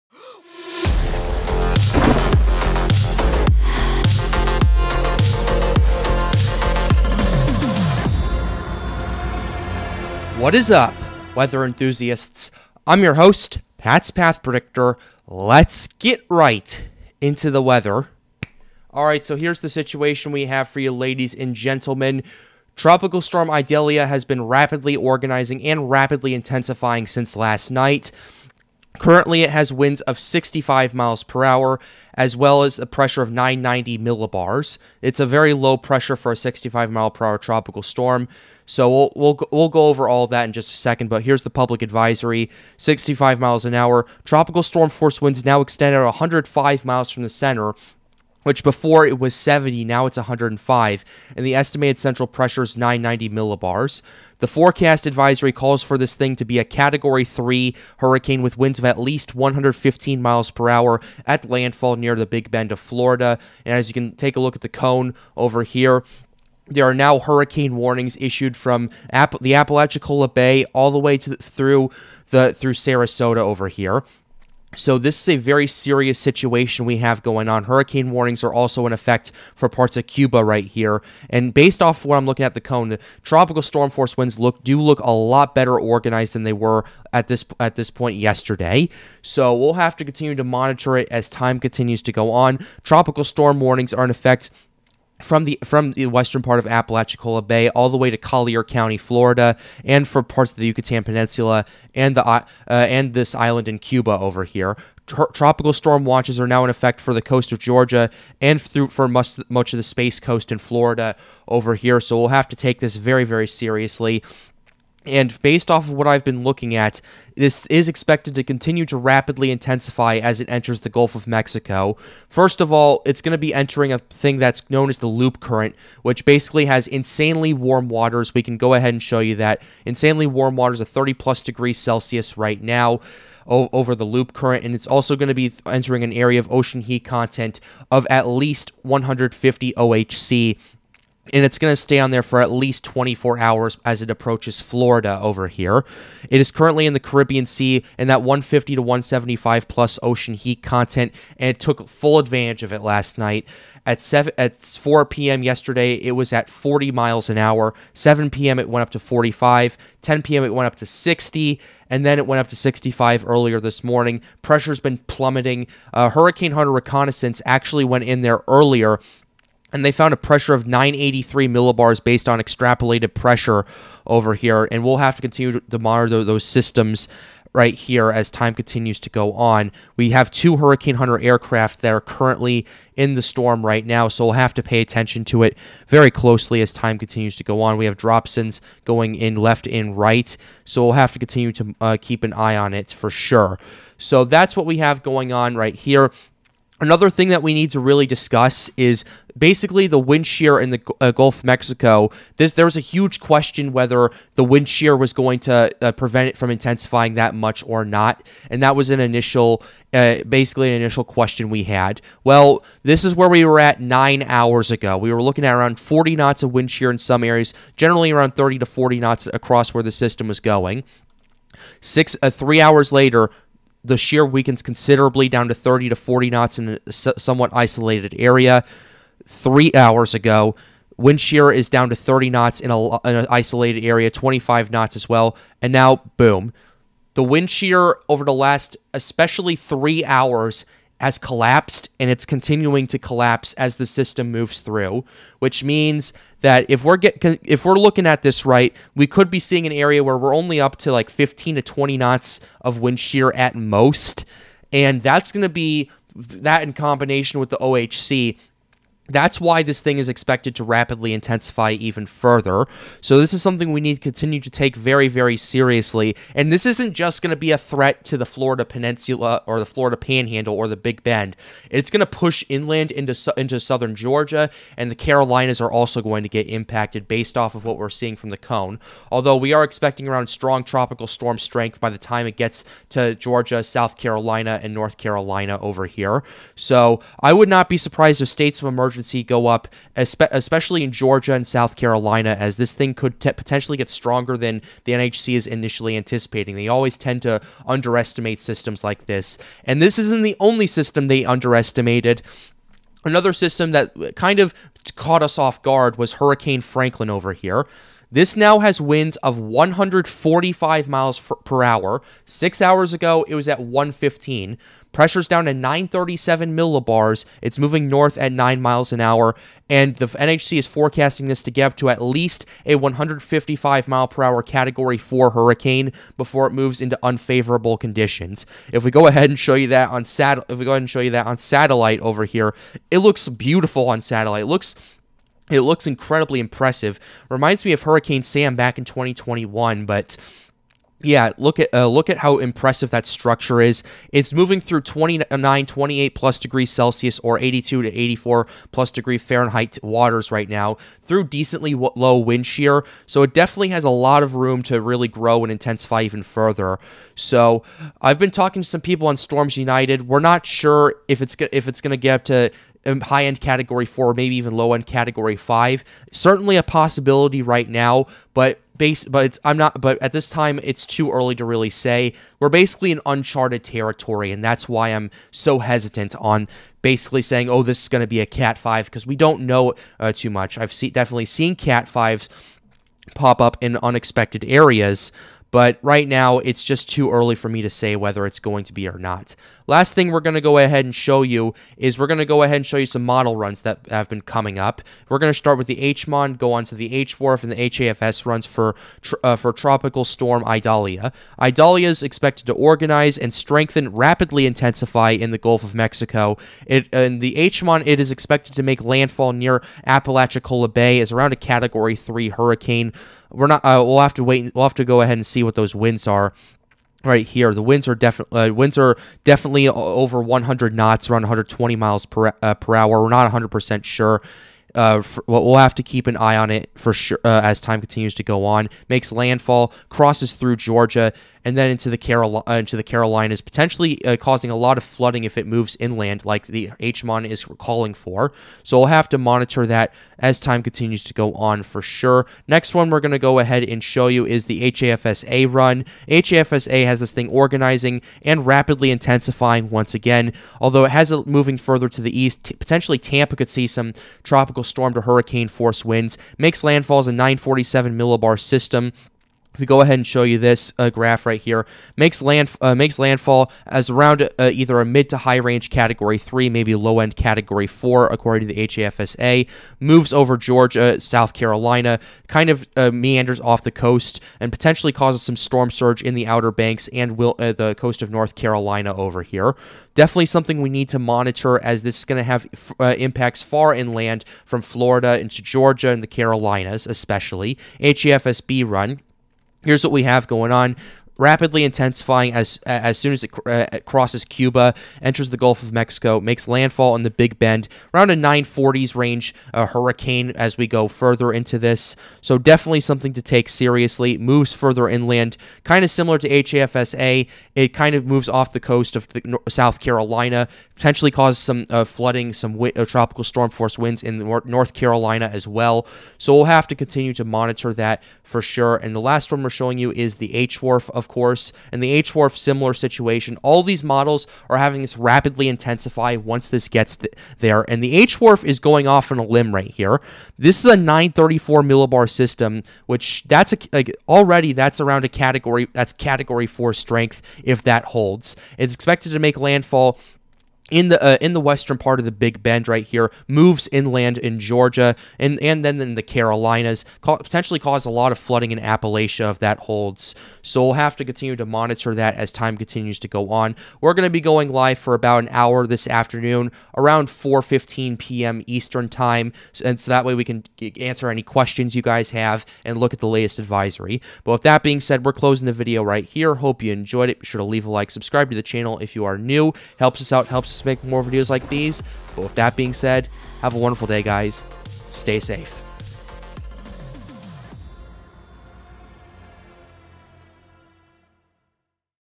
Tropical Update: Idalia And Franklin Rapidly Intensify! Hurricane Warnings Issued In Florida!
Also, LISTEN TO LOCAL OFFICIALS, They know better than I do about preparations!

Tropical Storm Idalia Has rapidly intensified in the Yucatan Straight and is expected to impact Florida as a Hurricane! Currently has winds of 65 MPH and a pressure of 990 mb. Governor Ron DeSantis of Florida has declared a State of Emergency ahead of this tropical cyclone!

ANOTHER Tropical Wave entered the Atlantic Ocean and now has a 70% chance of formation while moving over the Main Development Region this week! This has really organized over the last 24 hours and could certainly develop in the next few days!

We are now looking at several tropical waves that have recently peaked my interest and the conditions surrounding them. Right now, the waters are the warmest they have ever been and the Ocean Heat Content has been absolutely insane. On the southern tip of Florida, at the Everglades National Park, a temperature reading of 100.2° was recorded! This is a US record for how warm the waters are in the Atlantic! Compared to July 2020, the OHC is much more expansive and will allow for much more fuel for tropical storms and hurricanes to form, especially in the Caribbean Sea and Gulf of Mexico!

NOAA just issued their new 2023 Atlantic Hurricane Season forecast! They are calling for an active hurricane season, matching what everyone else has been saying! They expect 14-21 named storms, 6-10 hurricanes, and 2-5 major hurricanes!

Colorado State University put out their latest prediction of the 2023 Atlantic Hurricane Season! It is now calling for an active season with 18+ named storms with 9 hurricanes and 4 major hurricanes! The Weather Channel recently updated their Hurricane Season forecast and are now calling for 20 named storms with 9 hurricanes and 5 major hurricanes! I will keep you updated!